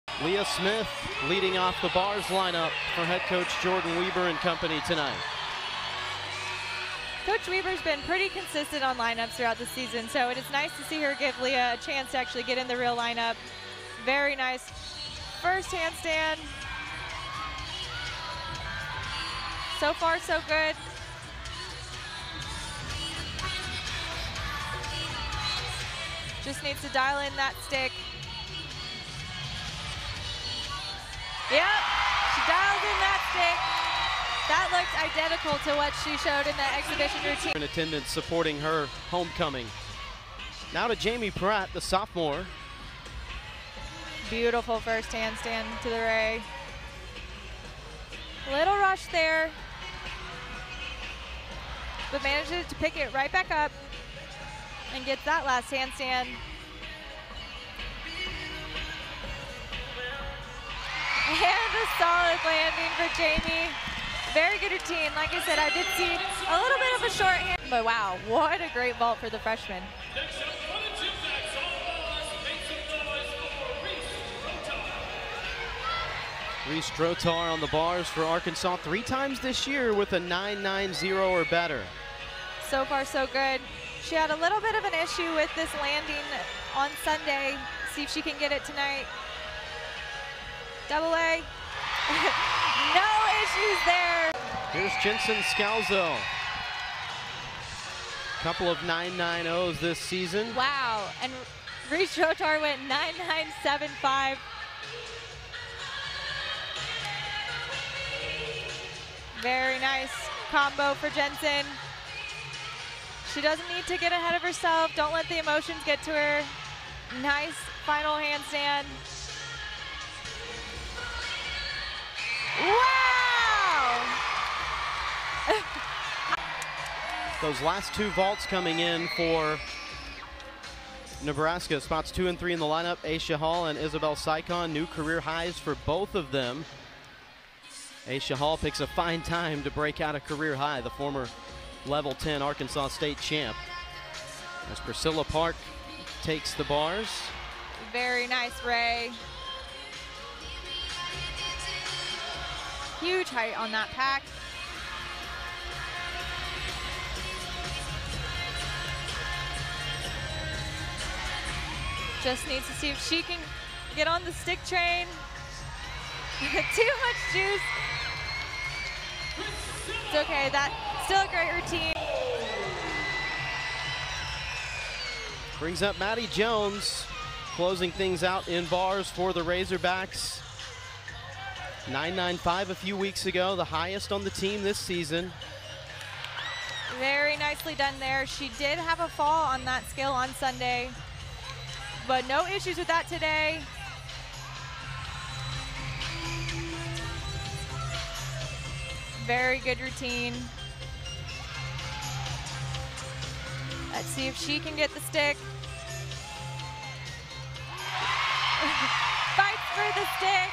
Highlights: Bars Versus Nebraska | RAZORBACK GYMNASTICS
The Arkansas Razorbacks hosted Nebraska at Bud Walton Arena for senior night.